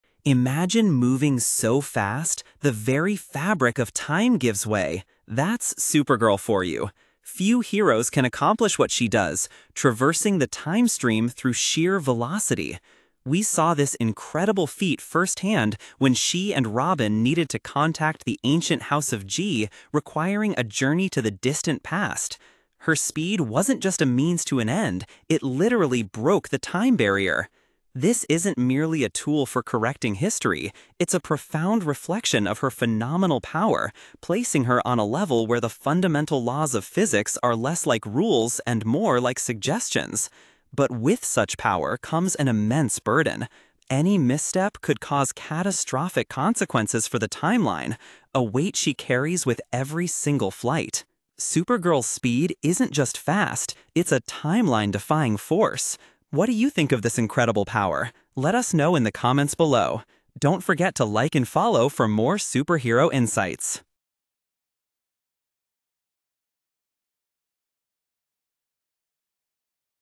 Supergirl: Her Speed Makes Physics Optional
Supergirl isn't just fast; she operates on a whole different plane of existence when it comes to speed. Ever wondered how the Maid of Might pulls off those impossible maneuvers that laugh in the face of gravitational pull and Newton's laws? In this video, we're diving deep into the phenomenal, often baffling, speed of Kara Zor-El. From supersonic rescues to seemingly instantaneous travel across continents, Supergirl consistently demonstrates that for her, physics is less of a rulebook and more of a mere suggestion. Get ready to have your mind blown as we explore how Supergirl truly makes physics optional!